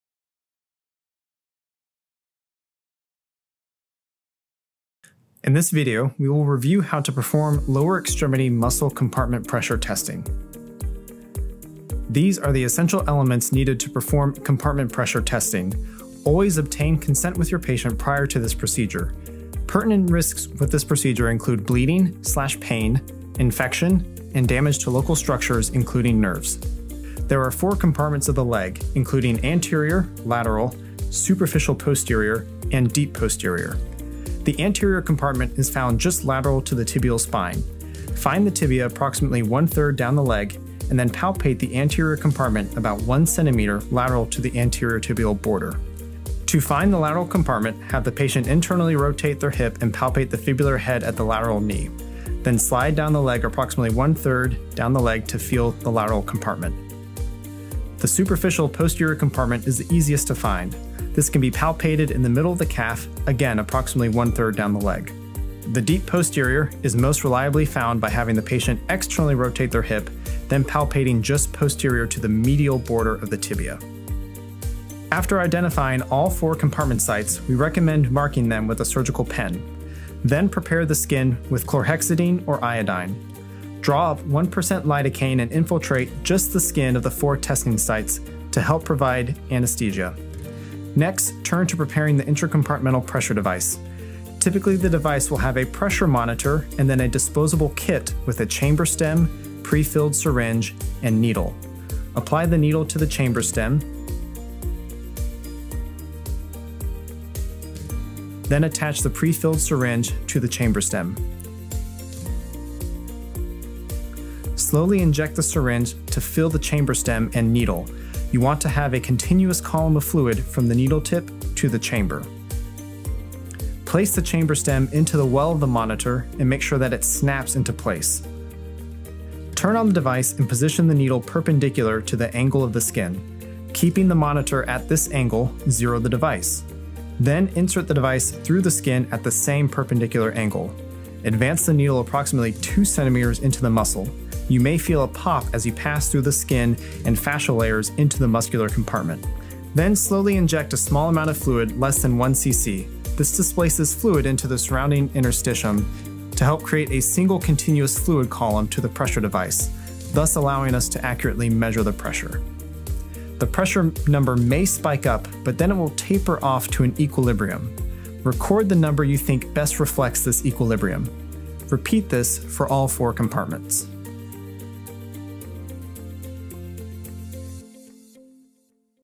How to measure lower leg compartment pressures.
Dr. Landon Mueller, EM and Sports Medicine Physician at MedStar Health, teaches how to measure lower leg compartment pressures using a stryker device.